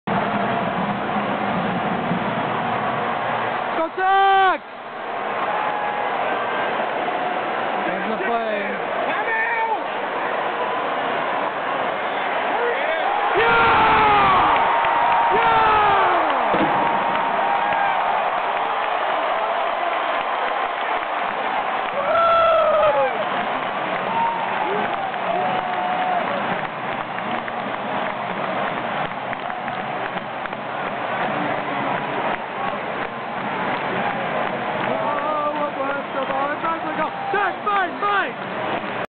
Potts throws TD to Batch
Taylor Potts throws the second TD of the game to RB Baron Batch to put the Red Raiders up 14-7